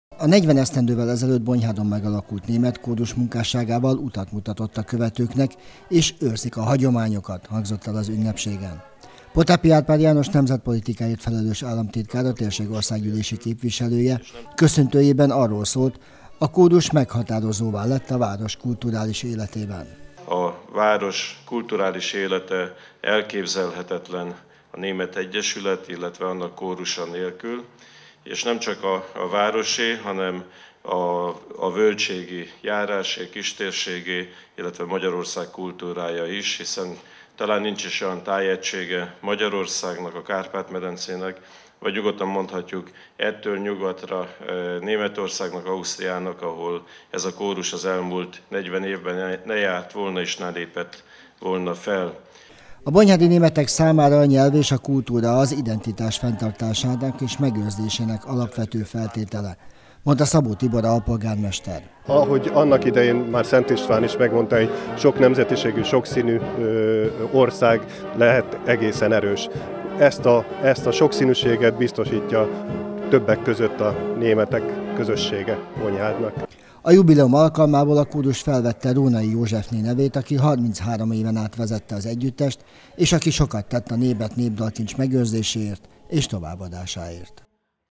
40 éves a Bonyhádi Német Kórus
Tolna megyében 40 esztendővel ezelőtt Bonyhádon alakult meg német kórus. Azóta az egyesület meghatározó a város kulturális életében és példaadó a térség hasonló szervezetei számára. A jubileumi ünnepségen részt vett Potápi Árpád János nemzetpolitikáért felelős államtitkár, a térség országgyűlési képviselője  is.